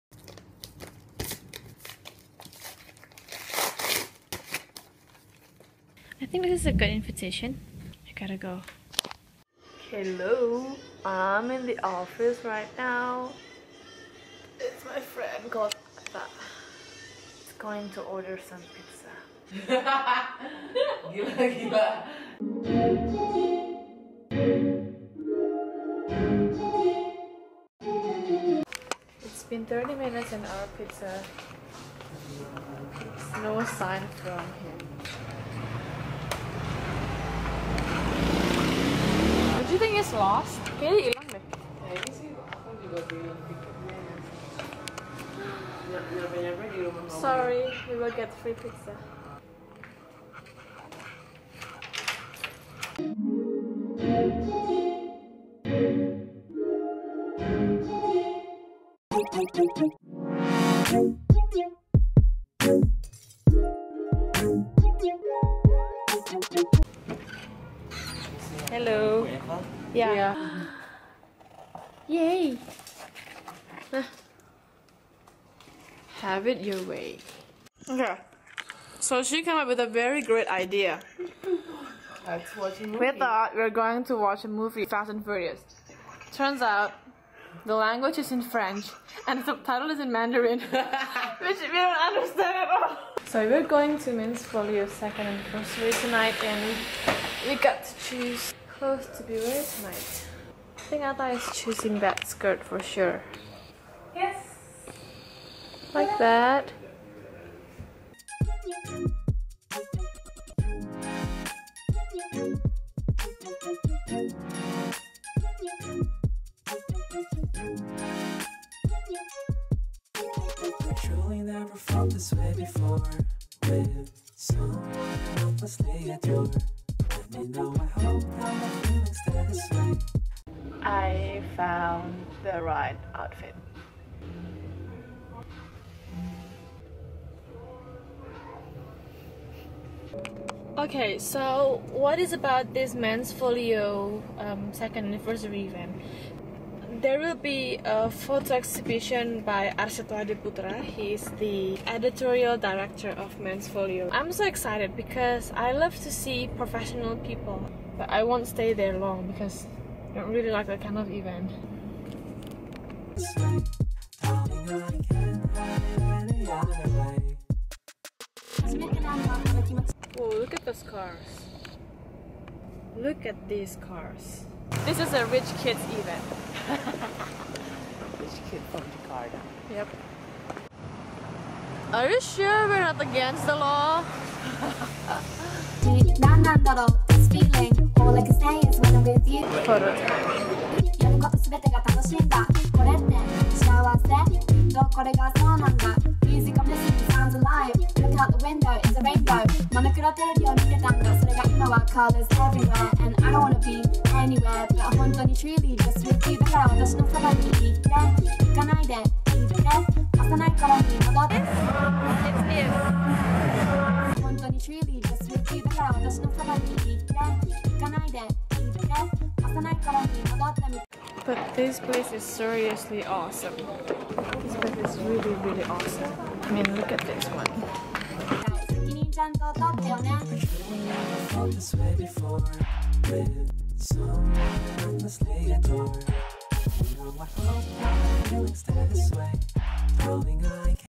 Daily Vlog| WE'RE NOT AGAINST THE LAW
it's Men's Folio Indonesia's 2nd anniversary event, the party was cool. it's only for cool kids, we're not a cool kids yet... But we're grateful to be able to come to that kind of party.